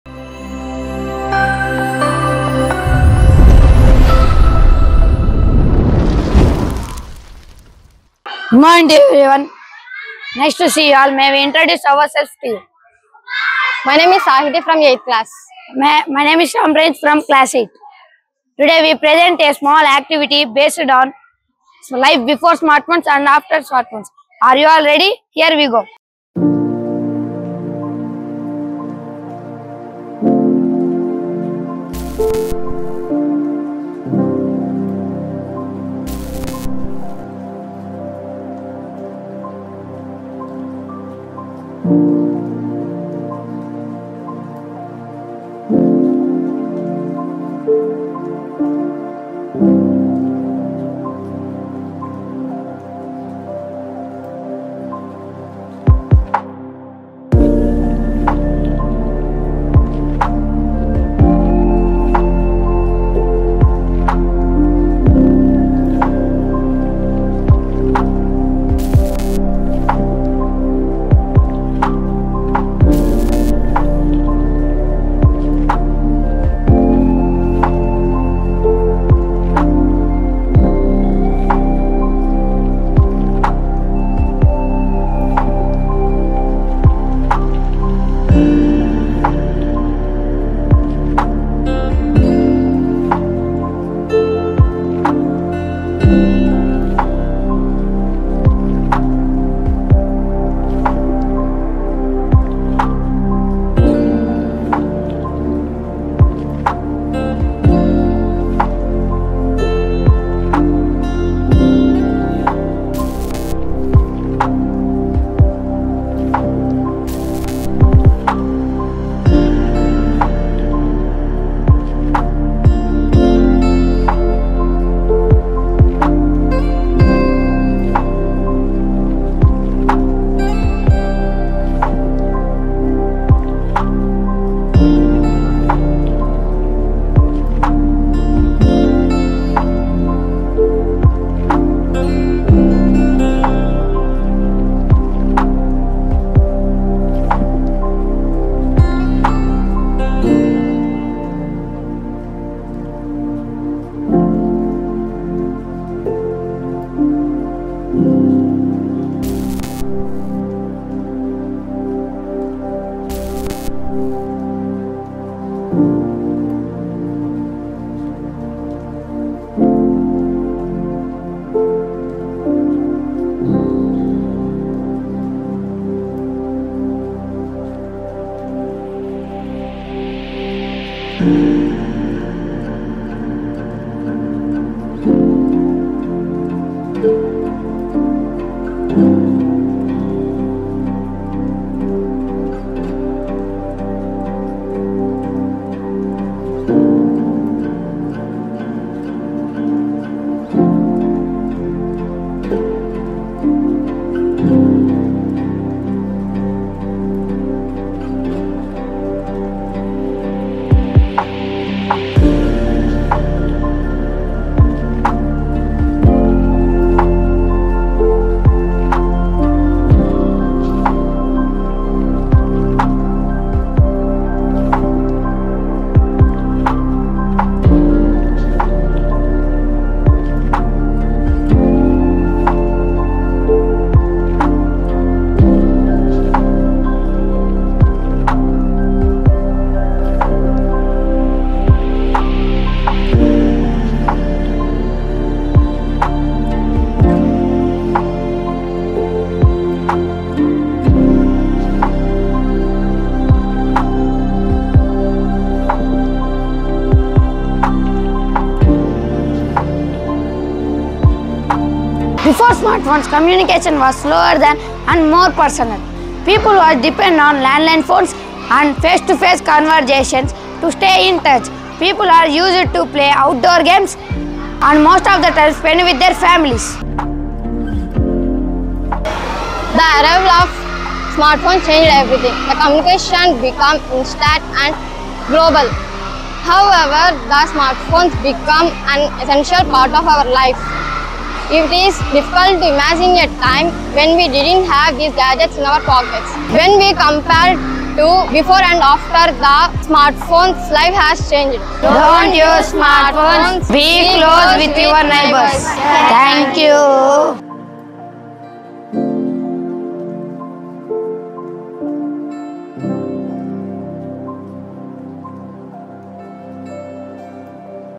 Sri Aurobindo School | A small skit on Before and After Smartphones
A small skit on BEFORE & AFTER smart phones by Sri Aurobindo School Students.